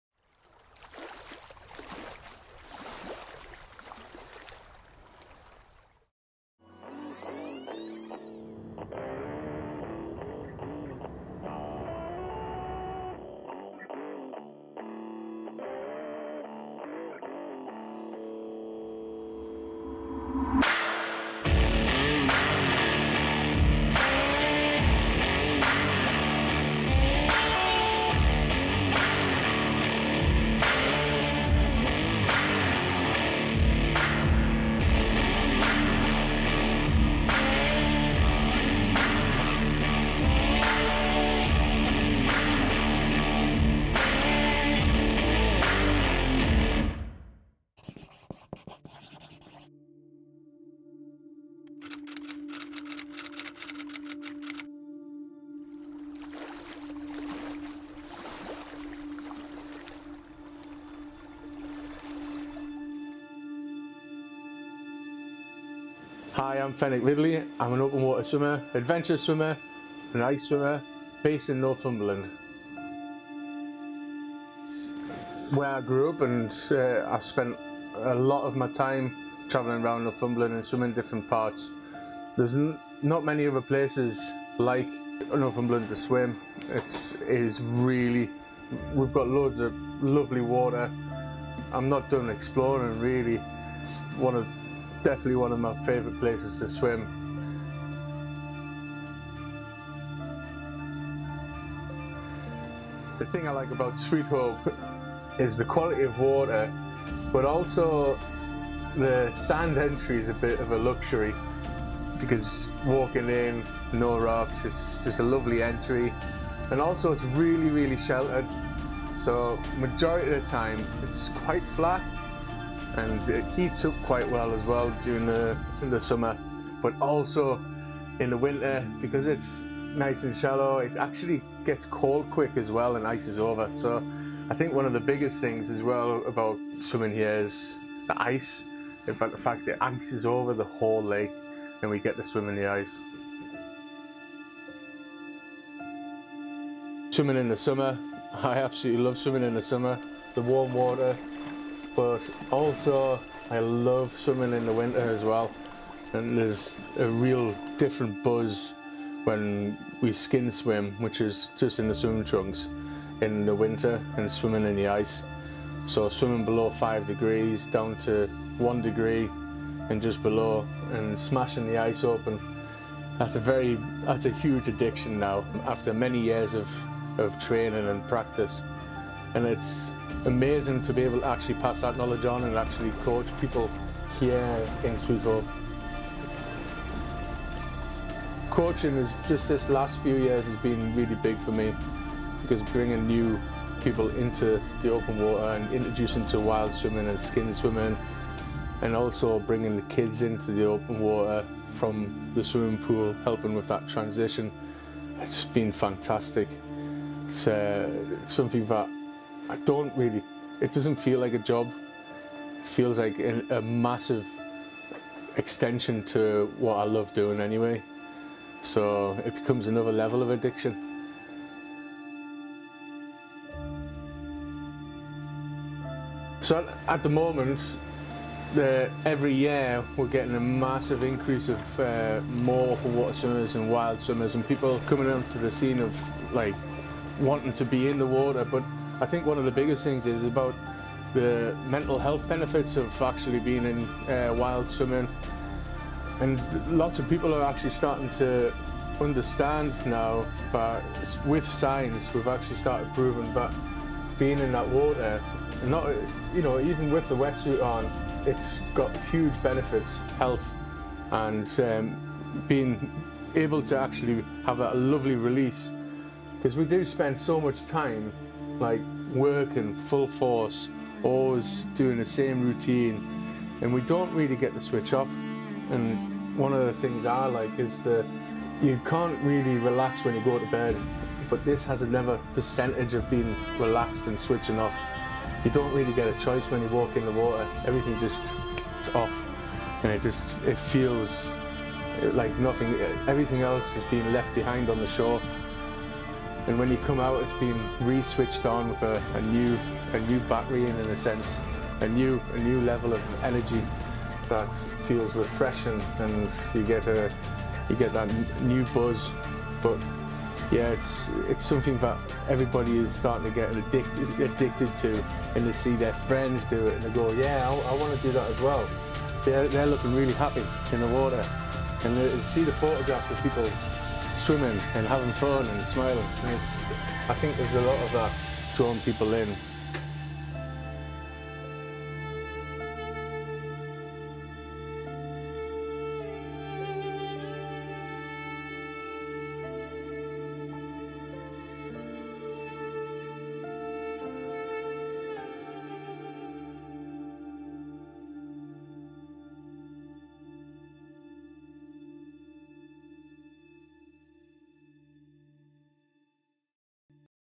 Fenwick Ridley, wild waters (by Chris Green)
EXPLR NLAND - Episode 4: Wild Waters
By Christopher Green Photography
My small interview with Chris